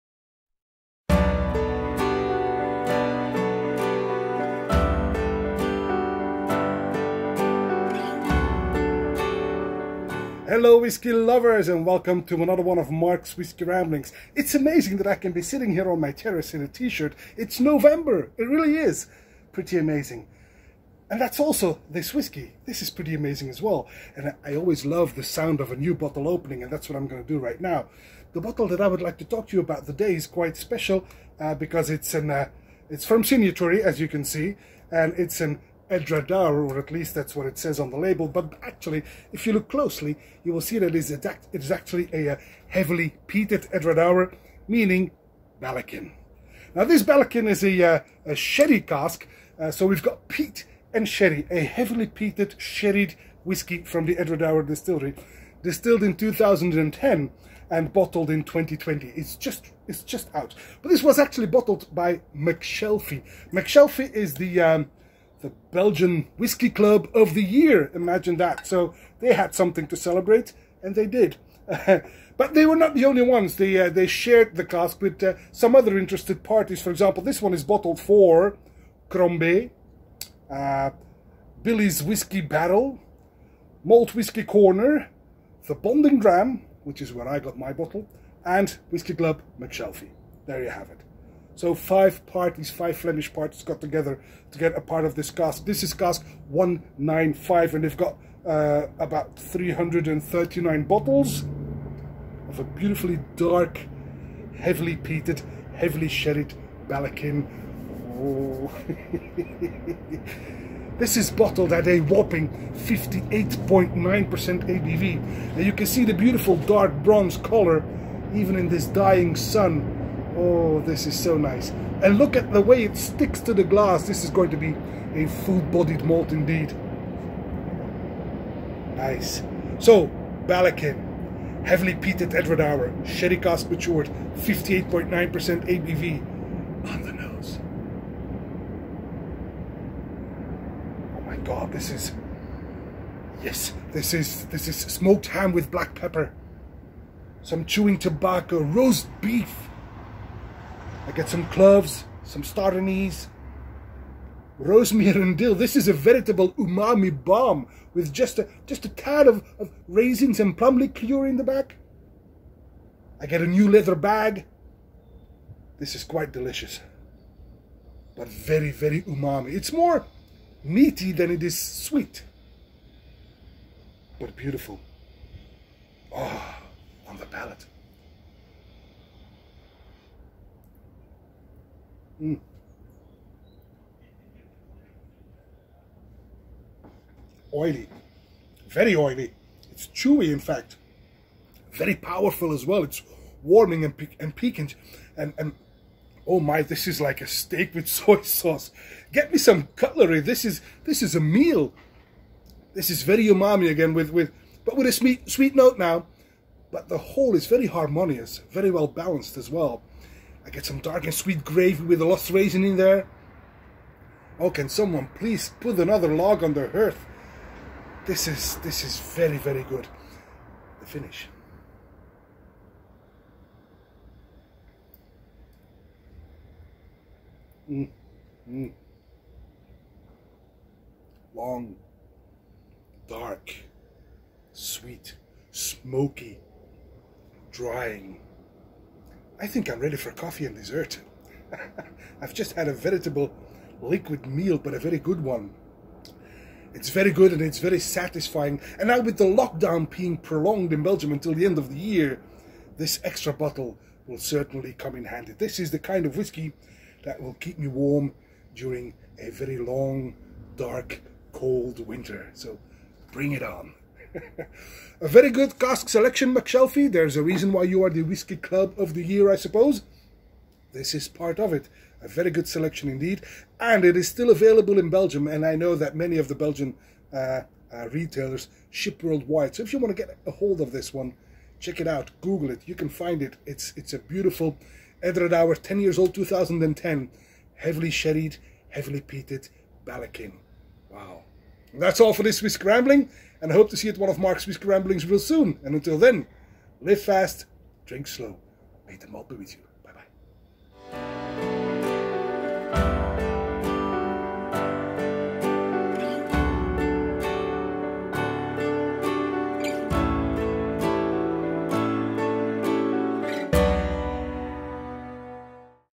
Mark’s Whisky Ramblings 354: Ballechin 10 Year Old 2010 Signatory for McShelfie
Mark Dermul, Belgian whisky vlogger, gets a taste of Pitlochry. Belgian Whisky Club of the Year McShelfie selected a single cask of 10 years old Ballechin that matured on a sherry cask. Indeed, it’s another heavily peated malt from the Edradour distillery.